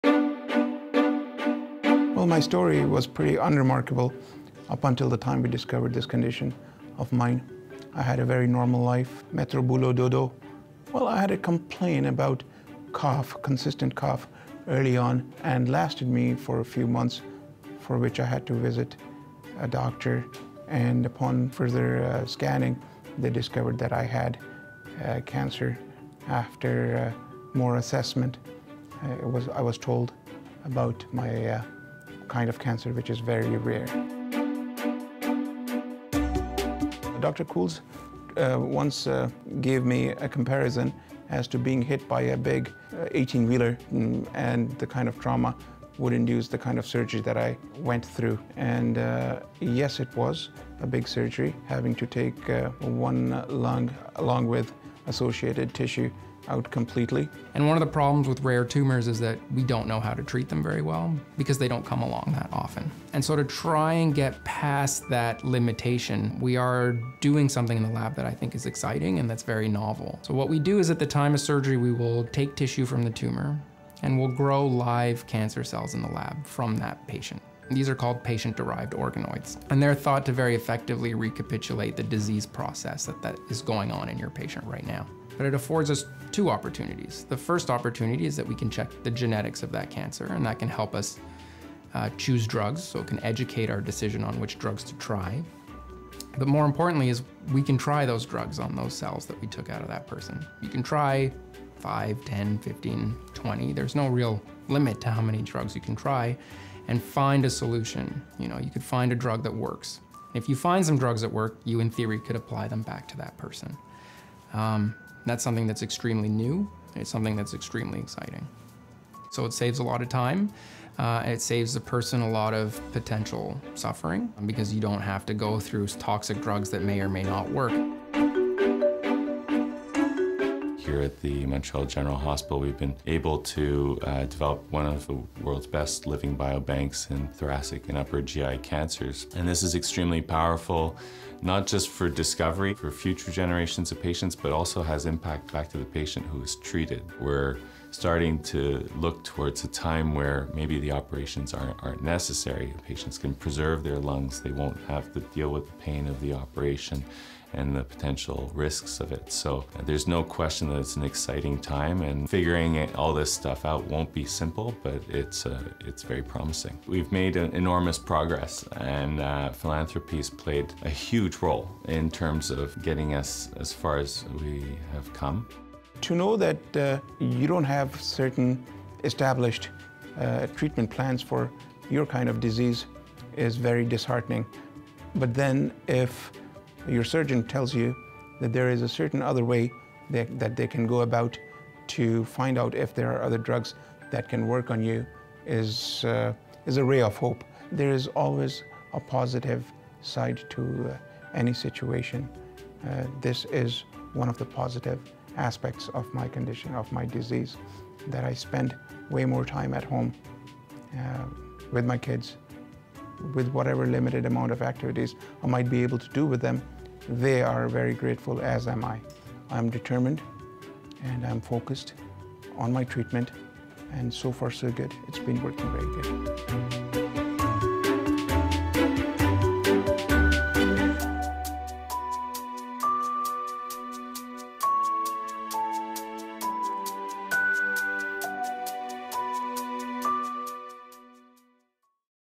Reinventing cancer care for our patients
Thoracic surgeons at the Montreal General Hospital have developed a living biobank to help find appropriate treatments for patients with rare tumors. At the time of surgery, they take tissue from the tumor and grow live cancer cells in the lab, to generate what they call patient-derived organoids. This new approach allows them to check the genetics of the cancer and to try multiple drugs on the organoid, all at once, saving both a lot of time and a lot of potential suffering for their patients. To learn more about innovation in oncology at the Montreal General Hospital, --- Les chirurgiens thoraciques de l'Hôpital général de Montréal ont développé une biobanque vivante leur permettant de choisir les traitements appropriés pour les patients atteints de cancers rares. Au moment de la chirurgie, ils prélèvent des tissus provenant de la tumeur et les cultivent en laboratoire, pour générer des organoïdes dérivés de patients. Cette nouvelle approche leur permet d'analyser la génétique du cancer et d'essayer plusieurs médicaments à la fois sur l'organoïde, sauvant beaucoup de temps et de souffrance potentielle à leurs patients. Pour tout savoir sur les innovations en oncologie à l’Hôpital général de Montréal, rendez-vous au